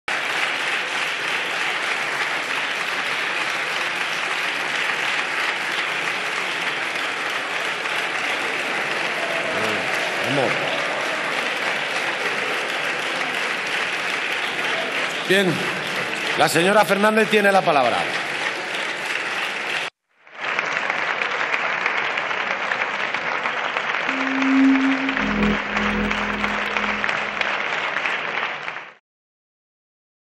Pablo Iglesias besa a Xavier Domènech
Pablo Iglesias y Xavier Domènech han celebrado con un beso en la boca su estreno en el Congreso, ante la mirada desconcertada de diputados del PP. Pablo Iglesias ha querido felicitar al líder de En Comú Podem por su discurso en el debate de investidura de Pedro Sánchez y lo ha hecho de la manera más efusiva posible.